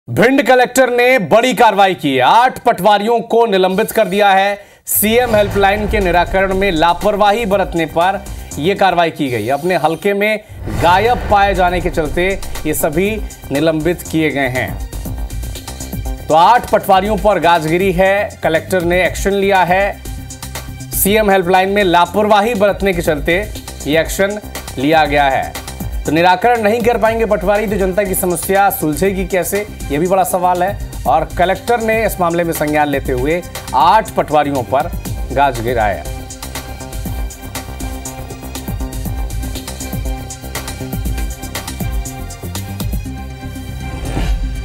Bhind News : 8 पटवारियों पर गिरी गाज | CM Helpline के निराकरण में लापरवाही का आरोप
IBC24, Bhind News, 8 पटवारियों पर गिरी गाज, CM Helpline के निराकरण में लापरवाही का आरोप